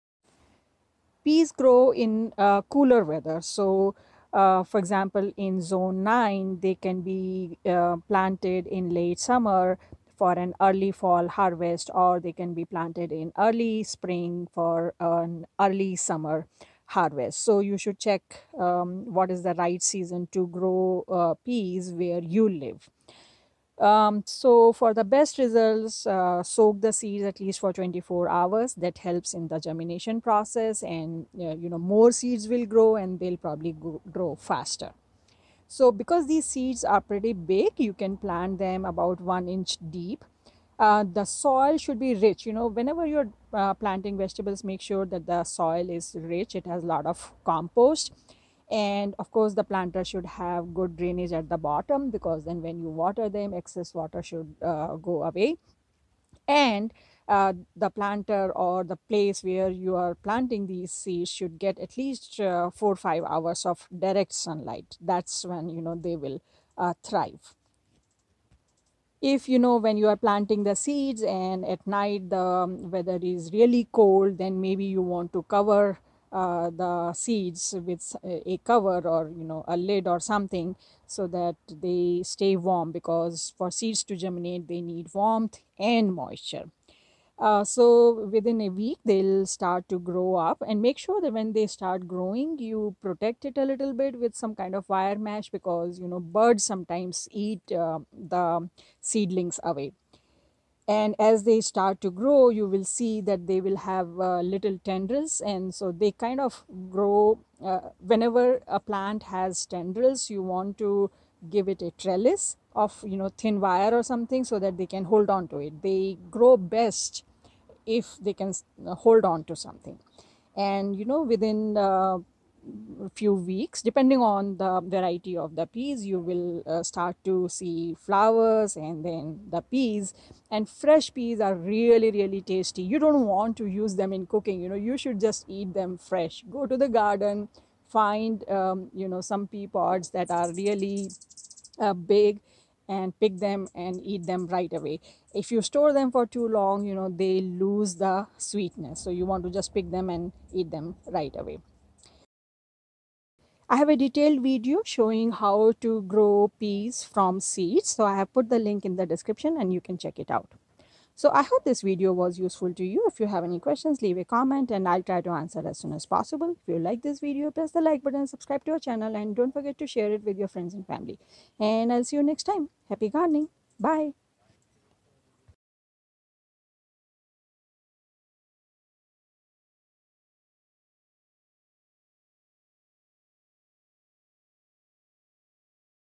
A quick guide to growing peas
Peas are a cooler weather vegetable. This is a quick video to show how you can grow peas from seeds. For a detailed video, check this out